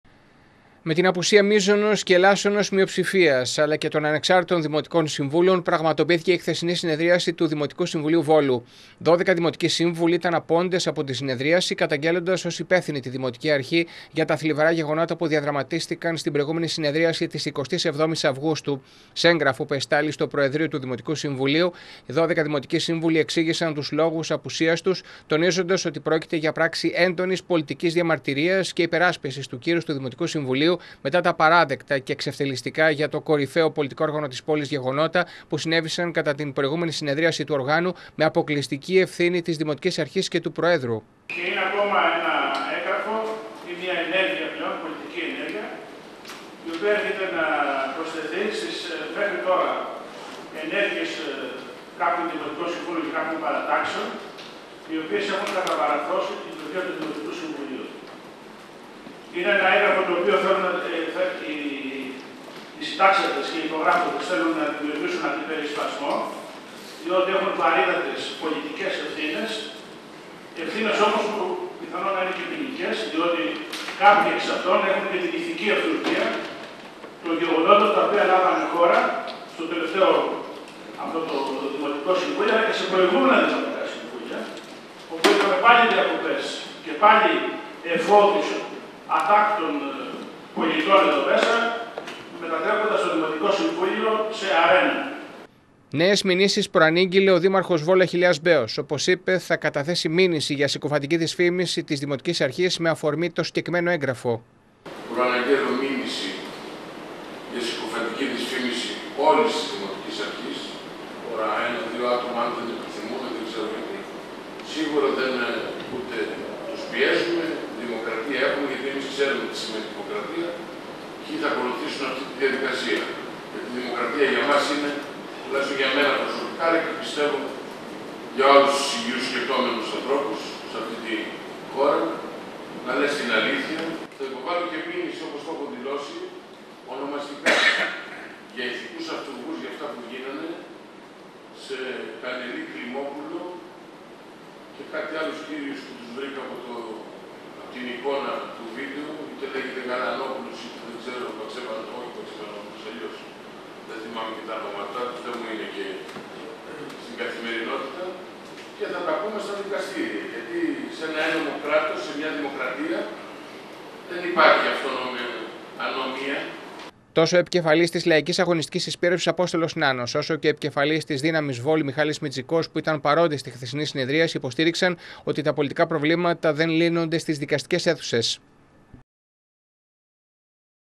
Βόλος Απείχαν από το δημοτικό συμβούλιο 180918
Με την απουσία των αντιπολιτεύσεων πλην των παρατάξεων της Λαϊκής Αγωνιστικής Συσπείρωσης και της Δύναμης Βόλου, έγινε η χθεσινή συνεδρίαση του Δημοτικού Συμβουλίου, καθώς με επιστολή τους δώδεκα δημοτικοί σύμβουλοι δήλωσαν ότι απέχουν ως πράξη έντονης πολιτικής διαμαρτυρίας και υπεράσπισης του Δημοτικού Συμβουλίου μετά τα «απαράδεκτα και εξευτελιστικά για το κορυφαίο πολιτικό όργανο της πόλης, γεγονότα, που συνέβησαν κατά την προηγούμενη συνεδρίαση του οργάνου». Με μηνύσεις προσωπικές αλλά και εναντίον όλων των αντιπολιτεύσεων απάντησε στην αποχή των αντιπολιτεύσεων ο δήμαρχος Αχιλλέας Μπέος, ενώ απαξιωτικά απάντησε στην επιστολή και ο πρόεδρος του σώματος, Γιώργος Μουλάς.